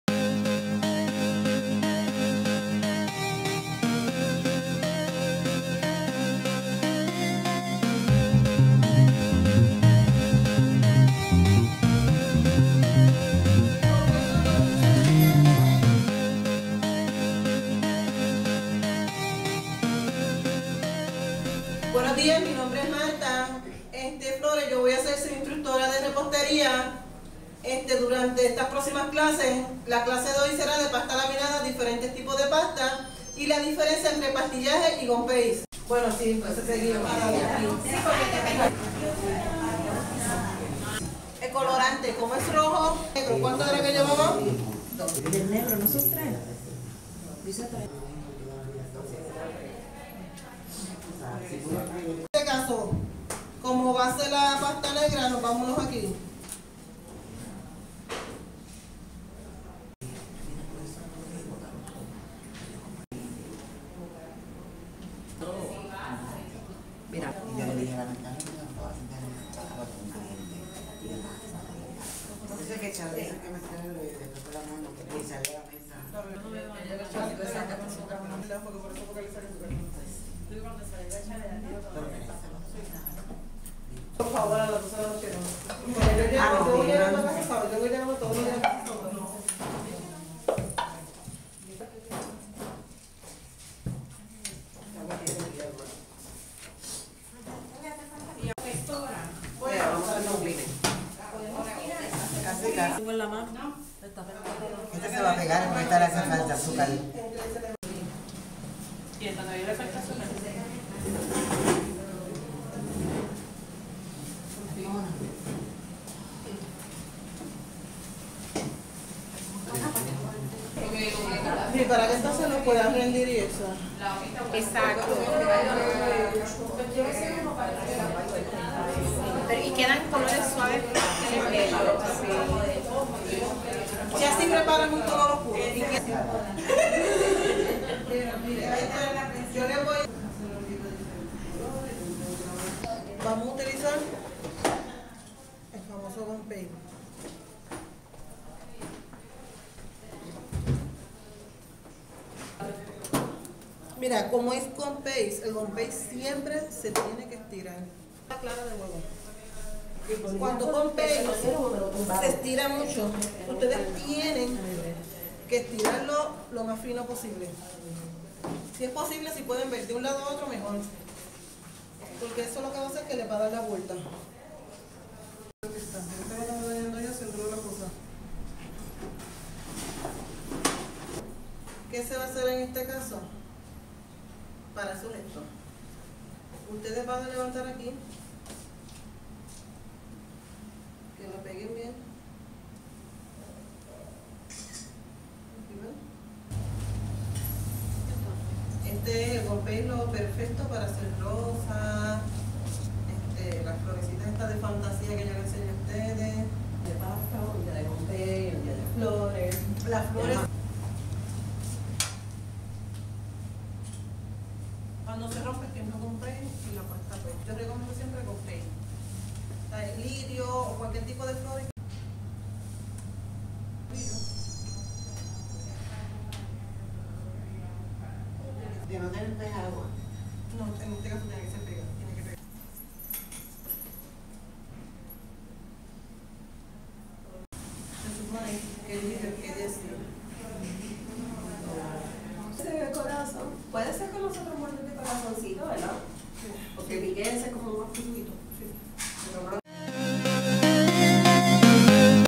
Baker's Market | Clases de Repostería | Juana Díaz | Puerto Rico
Podcast # 0147  AjiTerapia 01 Febrero 2017 | Baker's Market | Clases de Repostería | Juana Díaz | Puerto Rico
Show Cómico Músico Teatral, Podcast AjiTerapia, por Grupo AjiTerapia, en español desde Puerto Rico.

Clases de repostería en Baker's Market, Juana Díaz, Puerto Rico. Su dueña, Joselyn Valentín Traverso, tiene facilidades para que usted tome us clase de repostería.

En estos cortes de video, vemos a Marta Flores instruyendo a once participantes en un sábado en la mañana.

Baker's Market | Clases de Repostería | Venta de materiales y equipos para la confección de bizcochos, postres y mucho más | Juana Díaz | Puerto Rico

Conversamos con Joselyn Valentín Traverso, dueña de Baker's Market en Juana Díaz, Puerto Rico. Su esposo, Juan Alexis Batista y dos hijos son parte y razón de Baker's Market. Macbeth Segarra es parte importante de la empresa, una empleada muy efectiva ayudando a cada cliente.

Jocelyn, hija de una familia emprendedora, ha establecido éste negocio como solución a la dificultad de acceso a los materiales frescos y de calidad, necesarios para la confección de productos en el campo de repostería. El pueblo de Juana Díaz y pueblos limítrofes, tiene ahora mejor acceso para estos productos.

Baker's Market provee clases básicas y avanzadas y esta disponible para clases a niños, algo que les están pidiendo algunos de sus clientes.

Joselyn comenzó a crear productos en su hogar y abrieron oficialmente en agosto 2016. Baker's Market esta en la calle Tomás Carrión Maduro # 58 en Juana Díaz.

Provee productos båsicos para crear tus bizcochos. Se enfatiza la calidad y frescura de los productos, algunos provenientes de productores locales.

Cursos de Repostería por Profesionales
Lisandra
Marta Flores
Carlos Beltrán
Se implementan ya clases para niños. Llama a Jocelyn para coordinar.

Ofrecen abundancia de materiales para el mercado local, todos de calidad y frescos en Baker's Market de Juana Díaz.

En Facebook Baker's Market

Vea la entervista en el podcast  0147 AjiTerapia  01 Febrero 2017 | Baker's Market | Juana Díaz | Puerto Rico